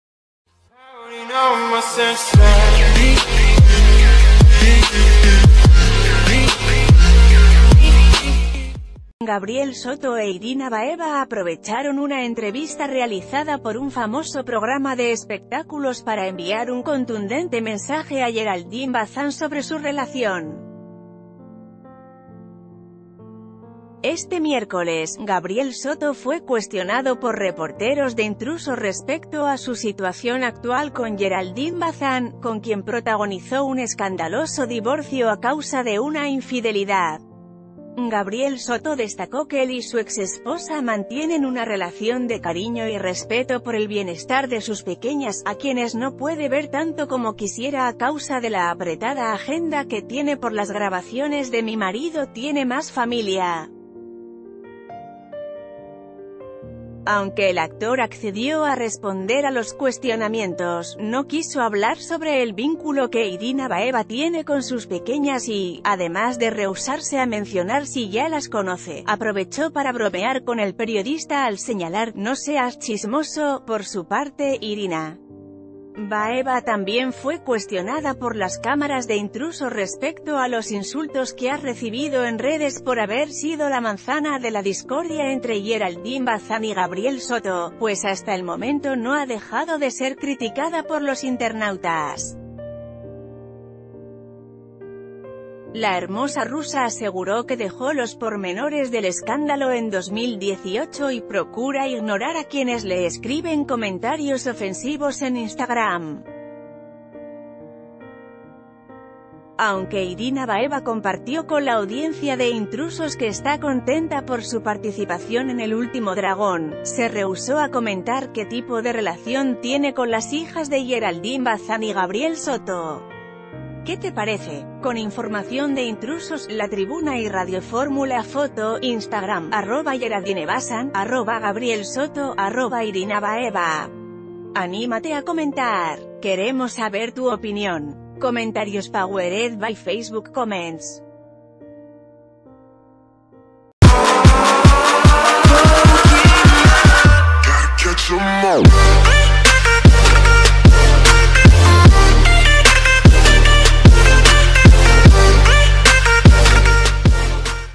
Gabriel Soto e Irina Baeva mandan contrastante mensaje a Geraldine Bazán
Gabriel Soto e Irina Baeva aprovecharon una entrevista realizada por un famoso programa de espectáculos para enviar un contundente mensaje a Geraldine Bazán sobre su relación. Este miércoles, Gabriel Soto fue cuestionado por reporteros de Intrusos respecto a su situación actual con Geraldine Bazán, ...